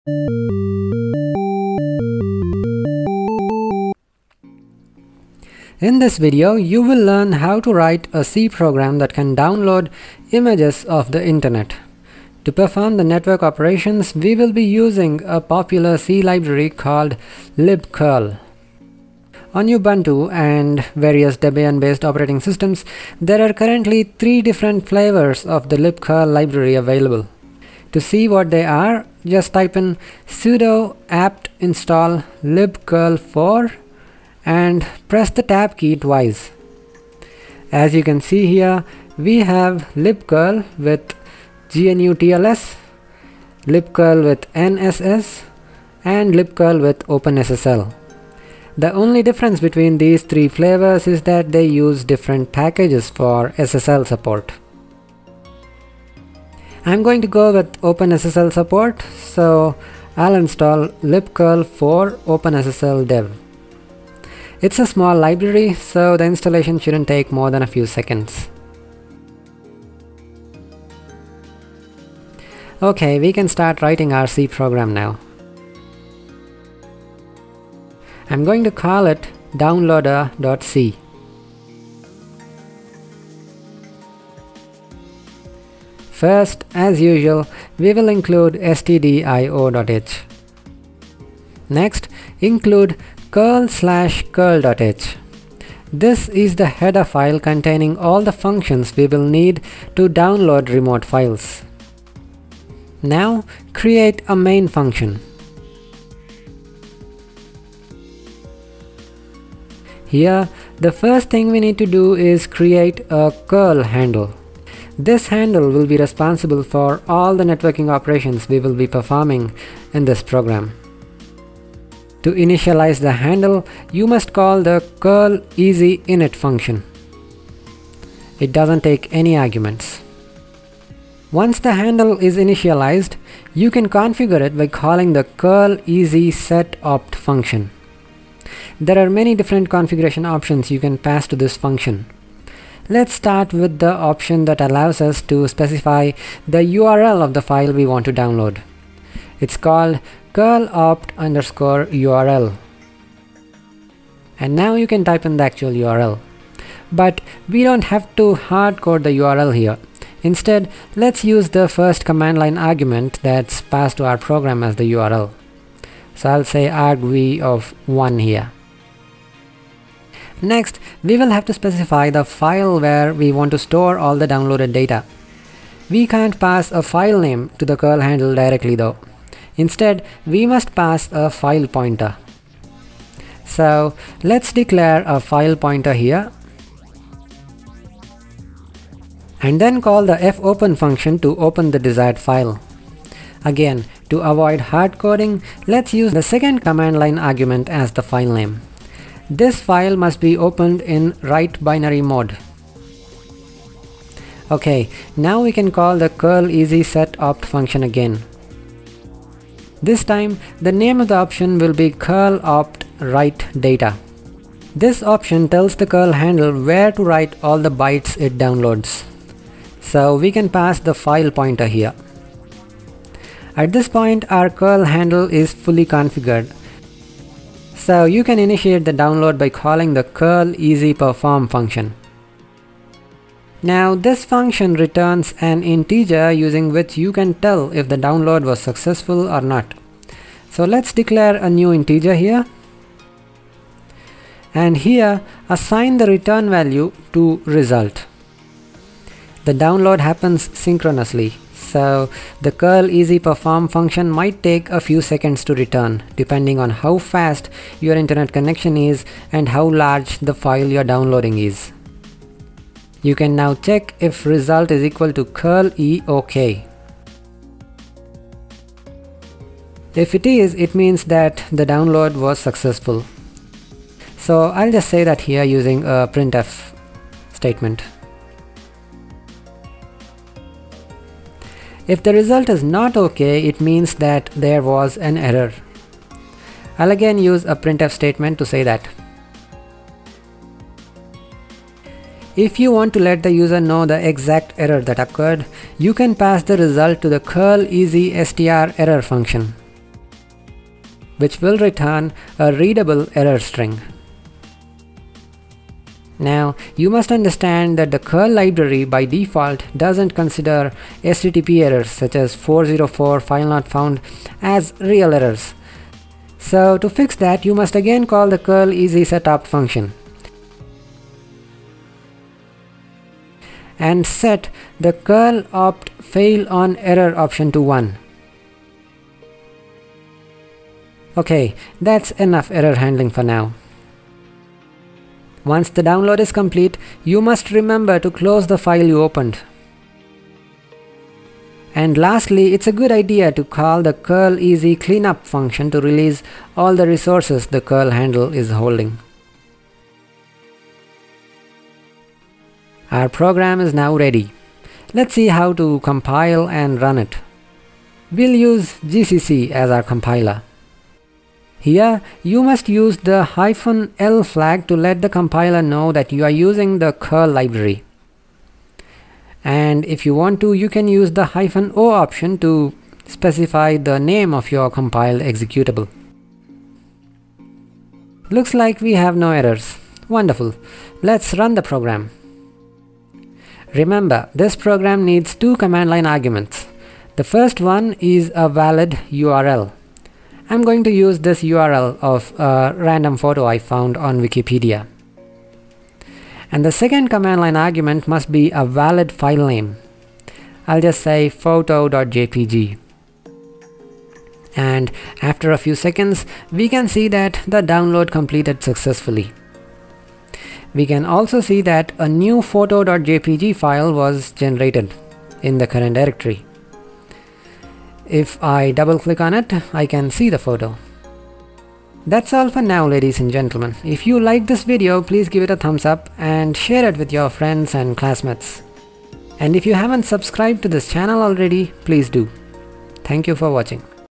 libcurl tutorial: Writing a C program to download files
In this tutorial, you'll learn how to use libcurl in C programs. In addition to showing you how to use the library to download remote files, I also show you the basics of error handling with libcurl.

Photos
Wallpaper by Casey Horner on Unsplash

Downloaded photo: Ximonic, Simo Räsänen [GFDL from Wikimedia Commons